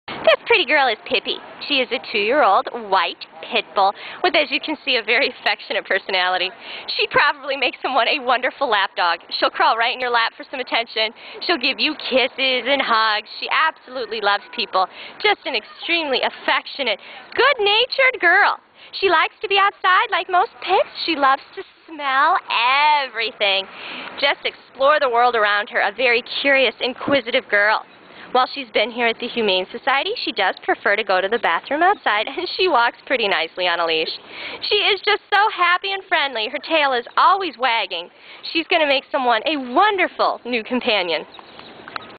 Pippi-Available at the Humane Society of Missouri
PIPPI - ID#A522882

I am a spayed female, white Pit Bull Terrier.
The shelter staff think I am about 2 years and 1 month old.
I have been at the shelter since Jan 13, 2012.